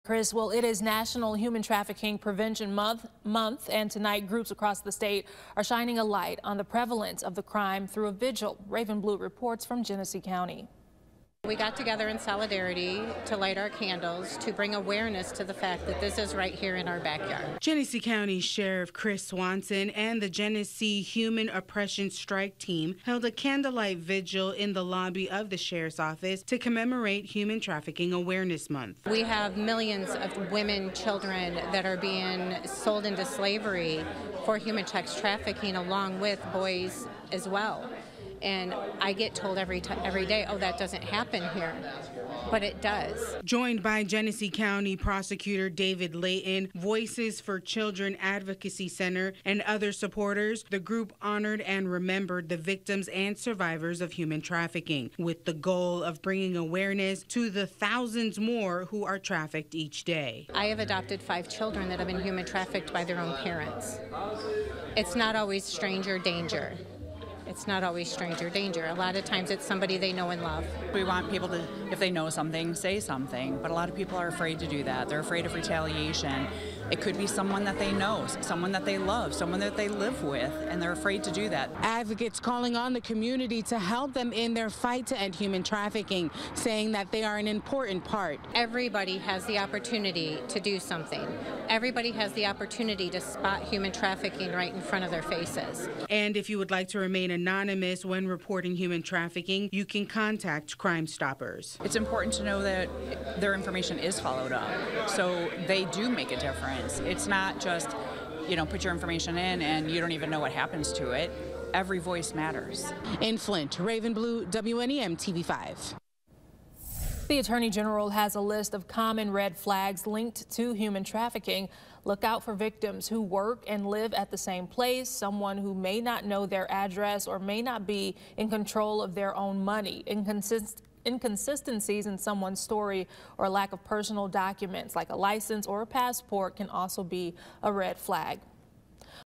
‘Every voice matters’: Vigil shines light on need for human trafficking prevention
On the last day of National Human Trafficking Prevention Month, groups across the state shined a light on the prevalence of the crime through a vigil.